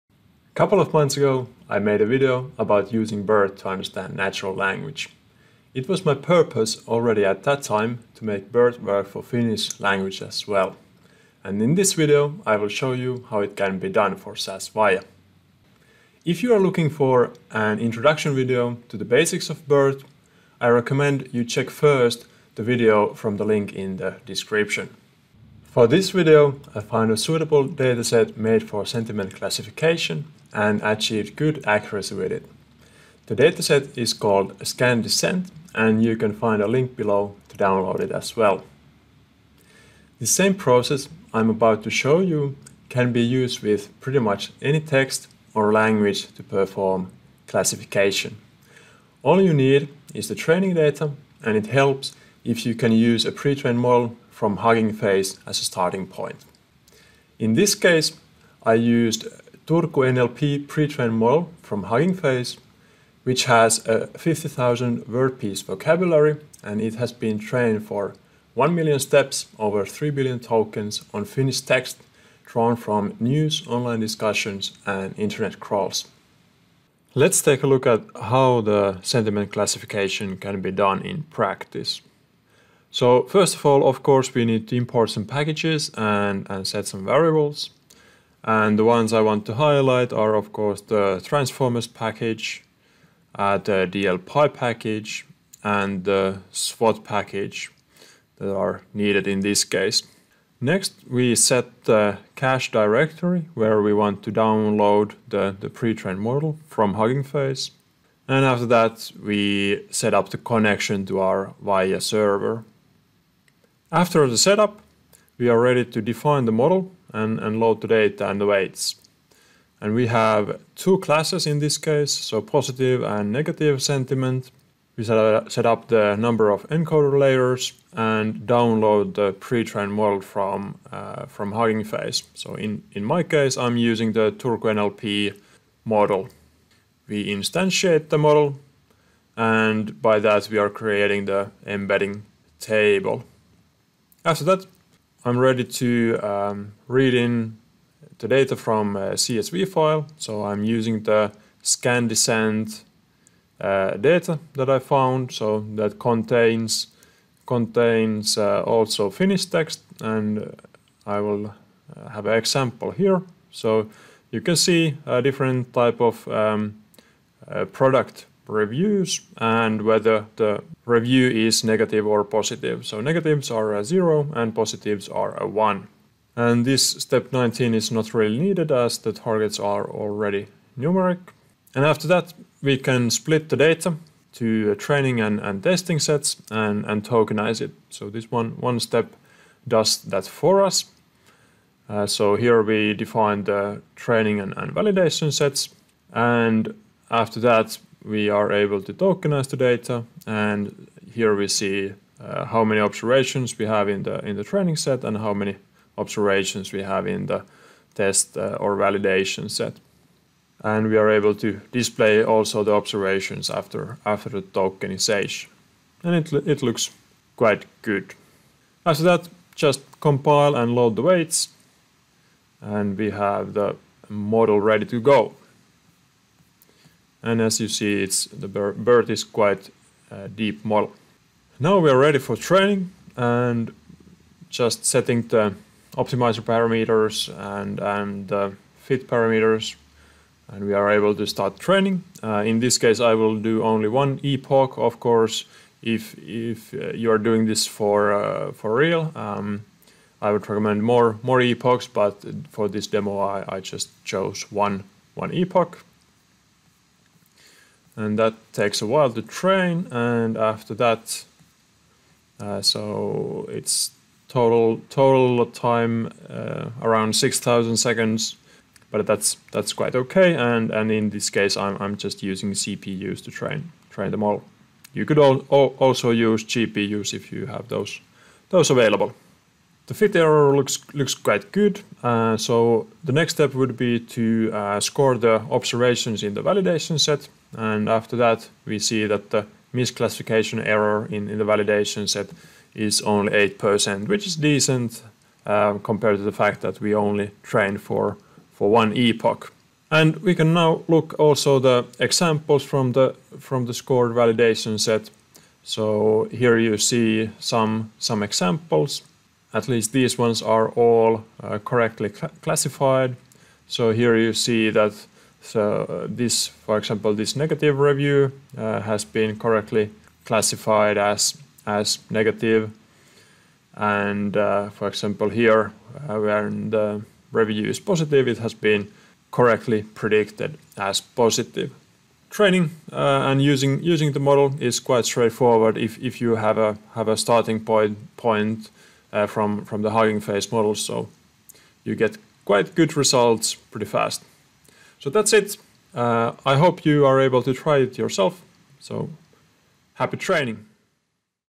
BERT for Text Classification on SAS Viya
In this video I show how to perform text (sentiment) classification with BERT models on SAS Viya. This example is for Finnish language, but the process is similar for other languages as well.